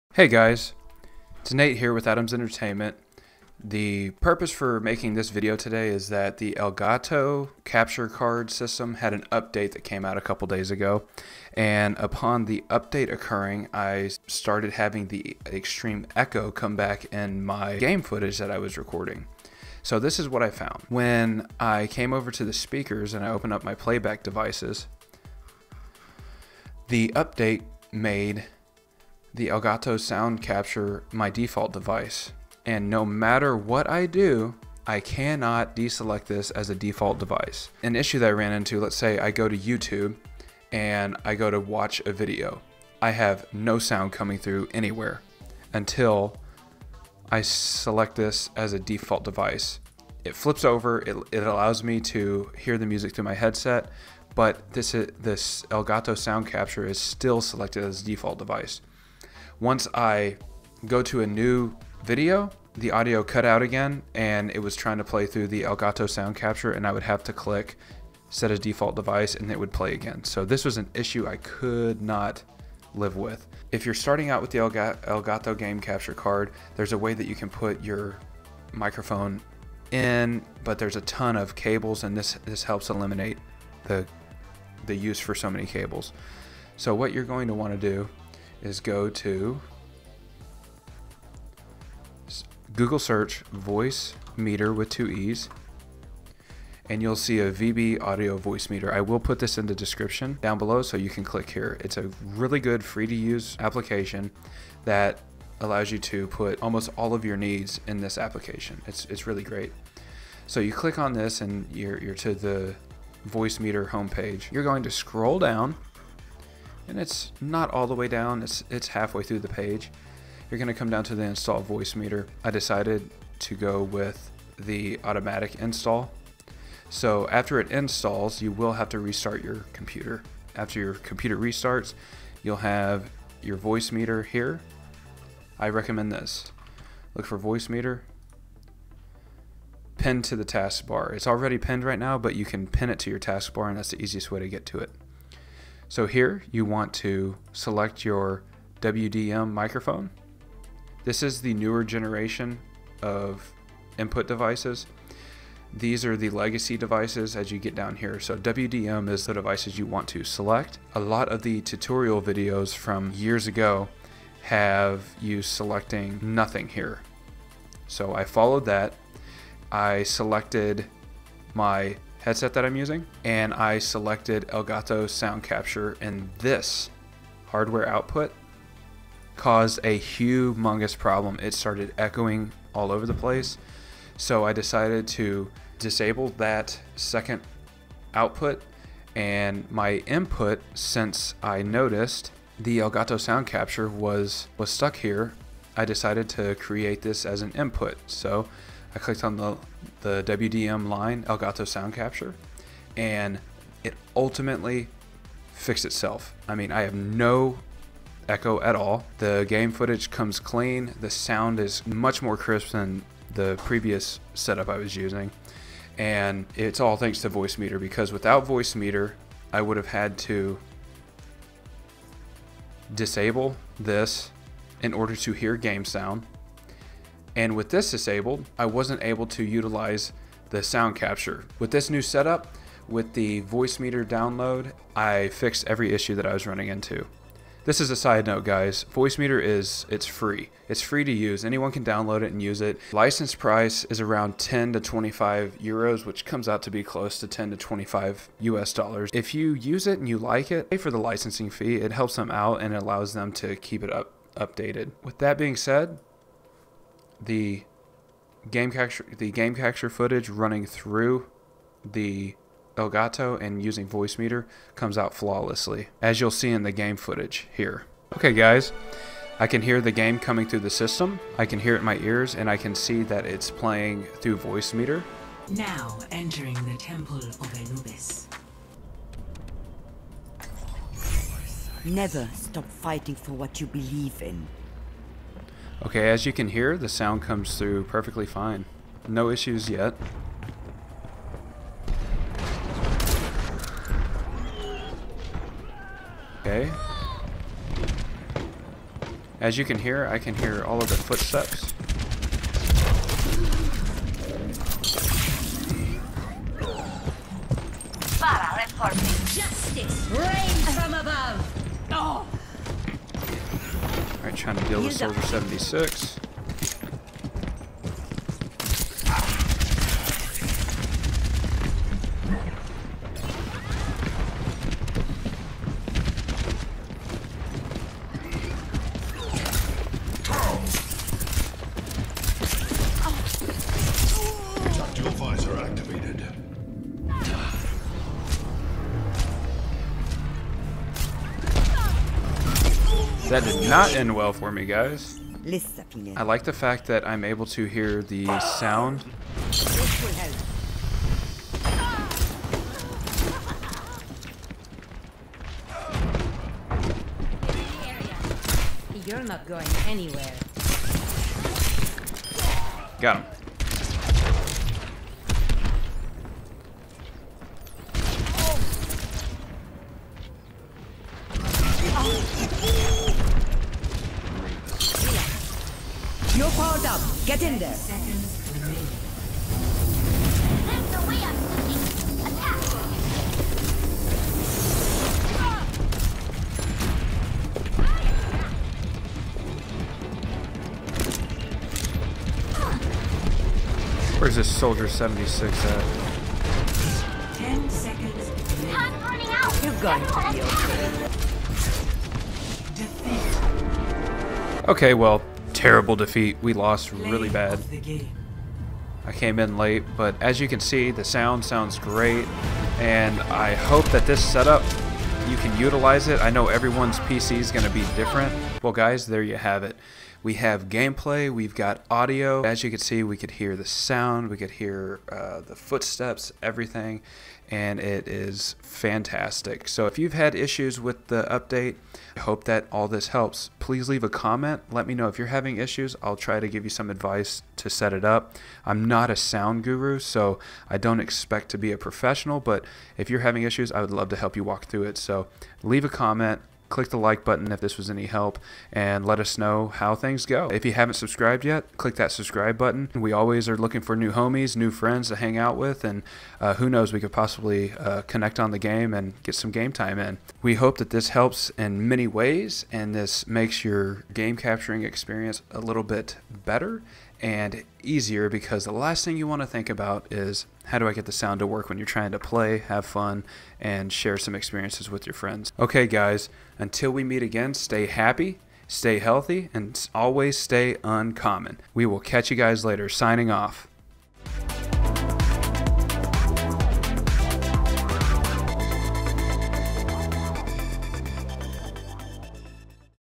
How to Set up Voice Meeter and Elgato
After the recent update Elgato released, my audio settings were changed and I had to reformat every input to get the device working again.  I recorded a step by step How to video for connecting the Elgato HD60S to the free application Voice Meeter.

I hope this helps if anyone has the Elgato and ran into the same problem, or if you are thinking about getting the Elgato.